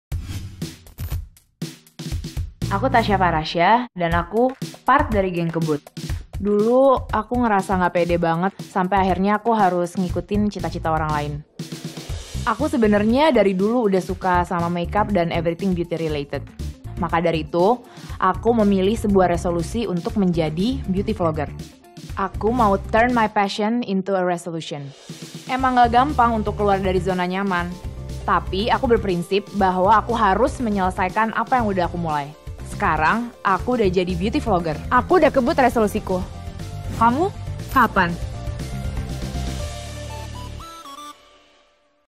GengKebut - Kebut Resolusi Ala Tasya Farasya
Tasya Farasya dan GENGKEBUT mau ngajak kamu kebut resolusi tahun ini sebelum terlambat! Cari inspirasi yang kamu perlukan di Tokopedia dan MulaiAjaDulu. Tasya udah kebut resolusinya, kamu kapan?